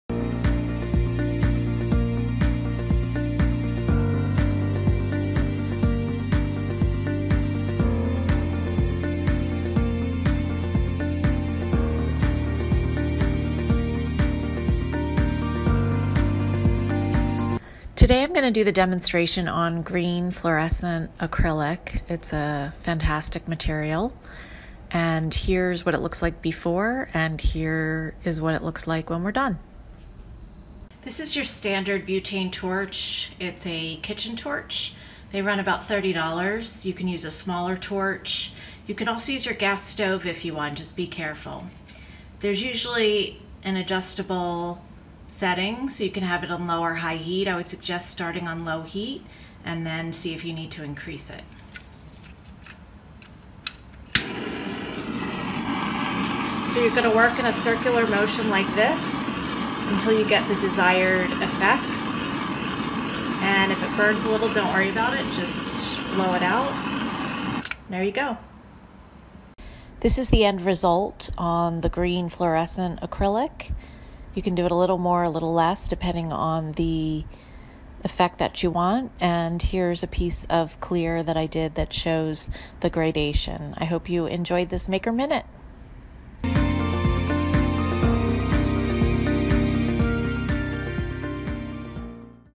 Make Acrylic Look Sandblasted in Seconds!
Acrylic is a strong and beautiful material for all your unique creations. Get inspired and build a one-of-a-kind creation in seconds using this torch sandblasting technique!